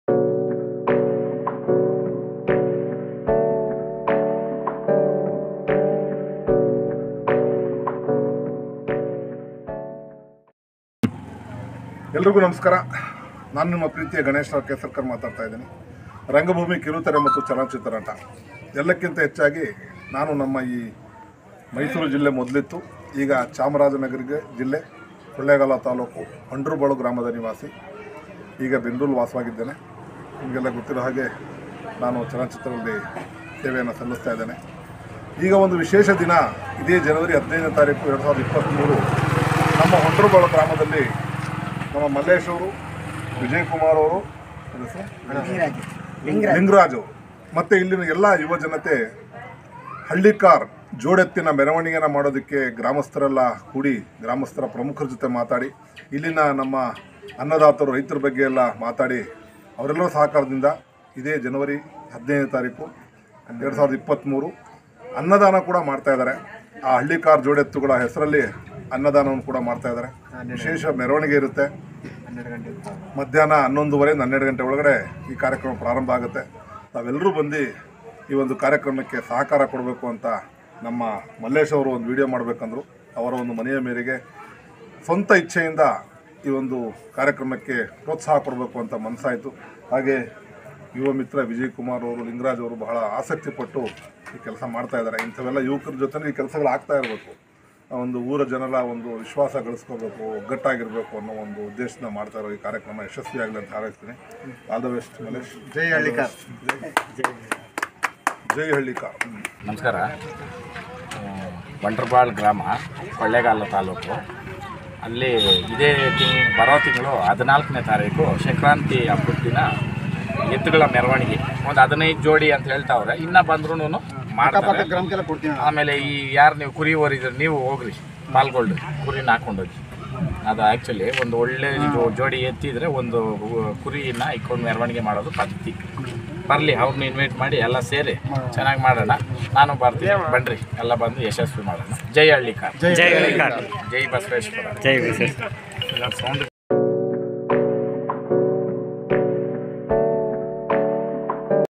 ಗಣೇಶ್ ರಾವ್ &ವರ್ತುರ್ ಸಂತೋಷ್ ರವರನ್ನು ಹಳ್ಳಿಕಾರ್ ಮಾಲೀಕರಾದ ಮಲ್ಲೇಶ್ ರವರಿಂದ ವಿಶೇಷ ಆಹ್ವಾನ
ಗಣೇಶ್ ರಾವ್ (ರಂಗ ಭೂಮಿ ಕಲಾವಿದರೂ&ವರ್ತುರ್ ಸಂತೋಷ್ ರವರನ್ನು ಹಳ್ಳಿಕಾರ್ ಮಾಲೀಕರಾದ ಮಲ್ಲೇಶ್ ರವರಿಂದ ವಿಶೇಷ ಆಹ್ವಾನ || ಕೃಷಿ ಕ್ರಾಂತಿ ||

ಇದೆ ತಿಂಗಳು 14ನೇ ತಾರೀಕಿನಂದು ವಂಡರಬಾಳು ಗ್ರಾಮದ ಗ್ರಾಮಸ್ಥರಿಂದ ಸಂಕ್ರಾಂತಿ ಹಬ್ಬದ ಪ್ರಯುಕ್ತ 15 ಜೋಡಿ ಹಳ್ಳಿಕಾರ್ ಎತ್ತುಗಳನ್ನು ಮೆರವಣಿಗೆ ನೆಡೆಸುತ್ತಿದ್ದಾರೆ. ಹೆಚ್ಚಿನ ಸಂಖ್ಯೆಯಲ್ಲಿ ರೈತರು ಆಗಮಿಸಿ ಕಾರ್ಯಕ್ರಮವನ್ನು ಯಶಸ್ಸು ಗೊಳಿಸಬೇಕಂದು ಗ್ರಾಮಸ್ಥರು ಕೇಳಿಕೊಳ್ಳುತ್ತಾರೆ.